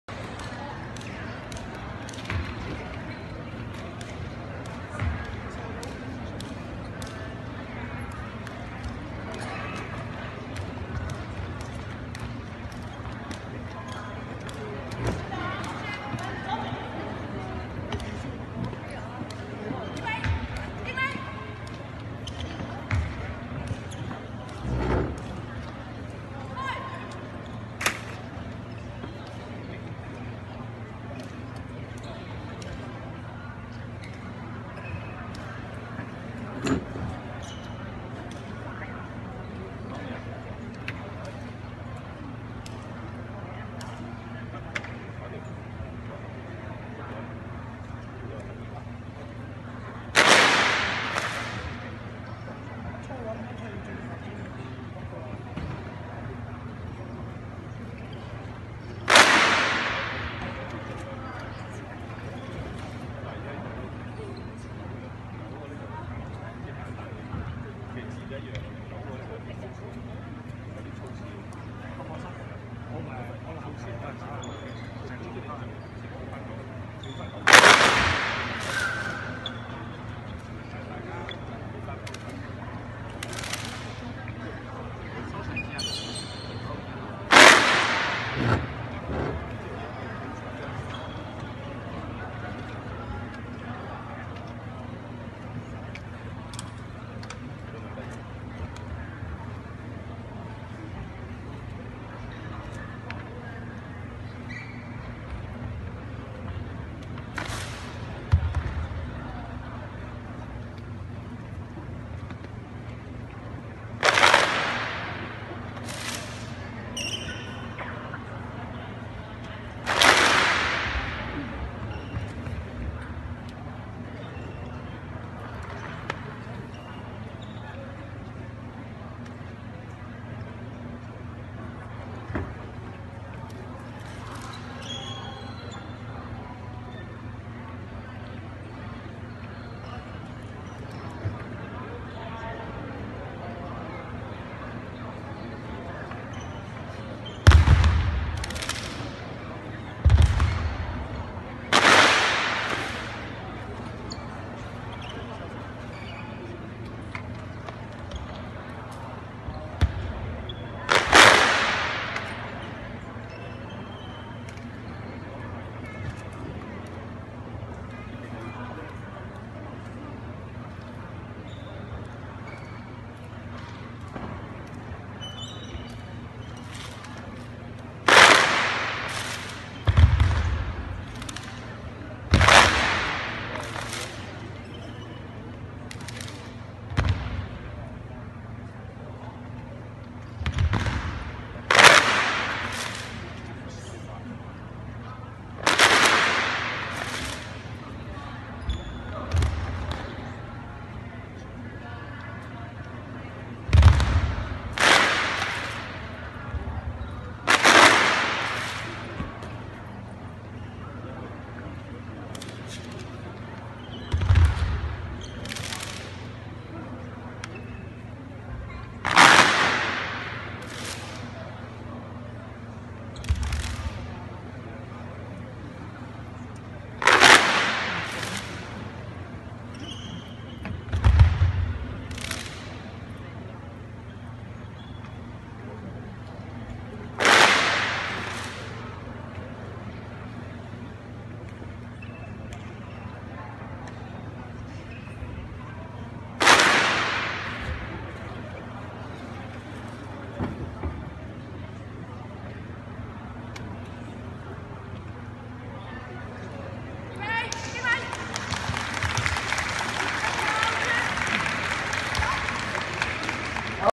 2018 國總 第二十六屆全港公開內家拳錦標賽 圑體賽 - 中華太極社 D隊 (優異獎) 陳式太極扇 Chen Taiji Fan Taichi
Master Feng Junli  China Taichi Club